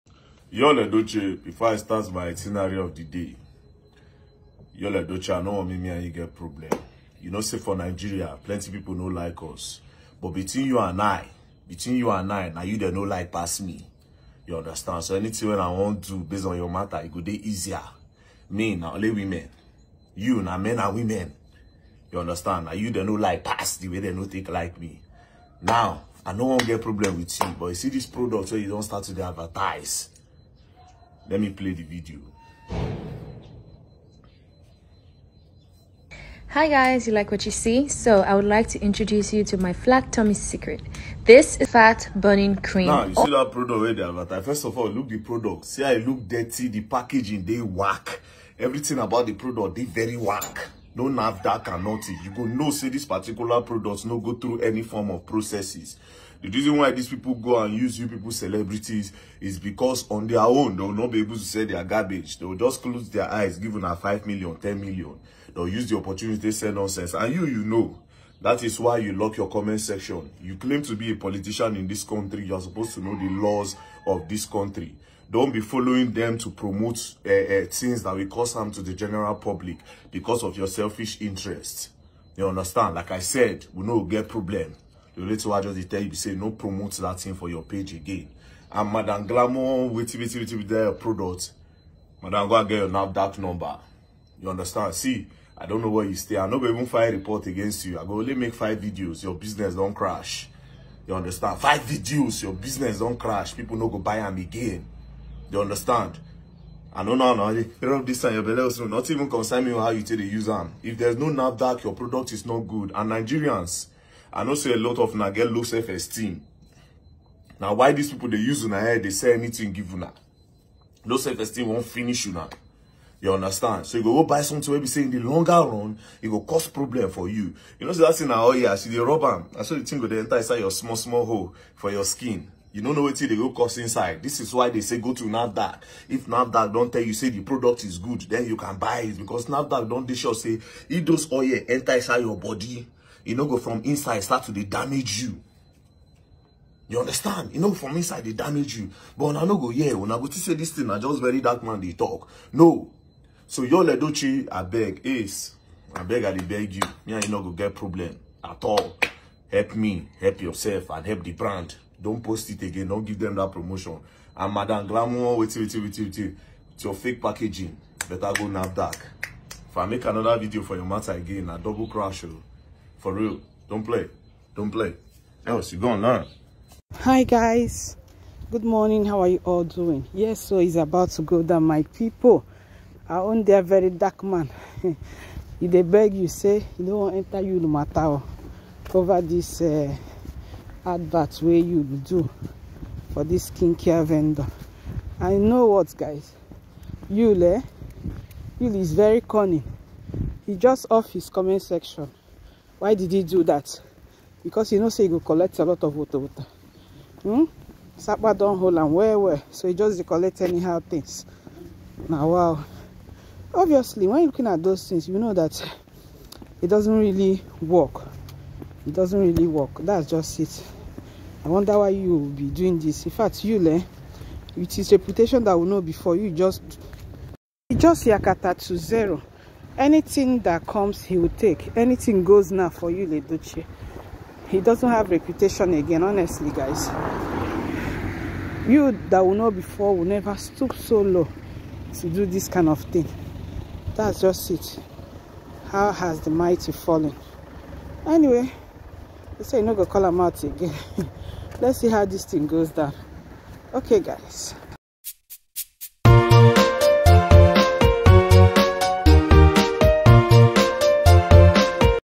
Its about to go down between Very Dark man and Yul Edochie over this advert he did for skincare ven
Its about to go down between Very Dark man and Yul Edochie over this advert he did for skincare vendor